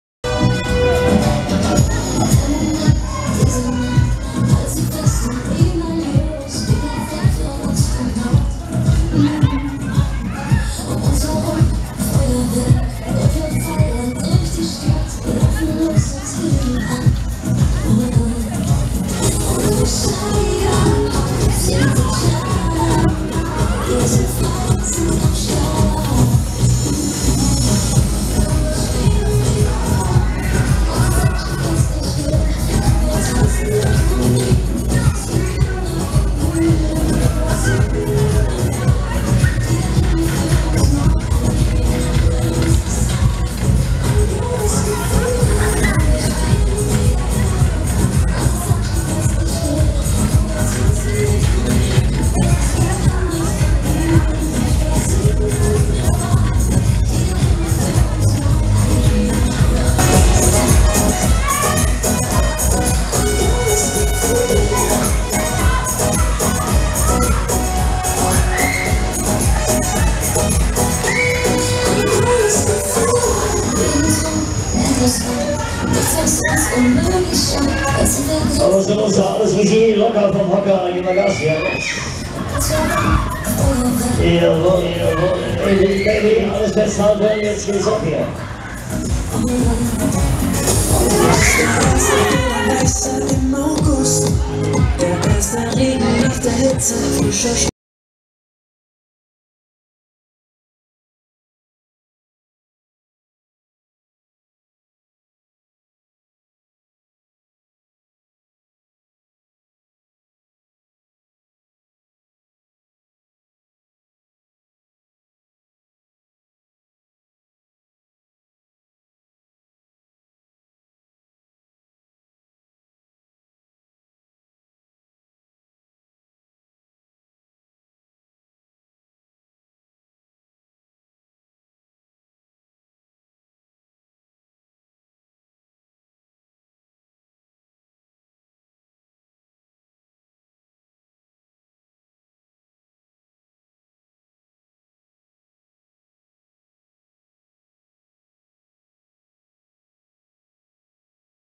Dschungel-Train - Scheele @ Wiesbaden Gibber Kerb 2017 (Offride)
Der "Dschungel-Train" von der Fa. Scheele auf der Gibber Kerb in Wiesbaden am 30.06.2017 (Offride)

Besitzer: Scheele, Kassel
Typ: Berg- und Talbahn/Musik-Express
Hersteller: Mack (D)
Kapazität: 60 Personen in 20 Chaisen
Baujahr: 1980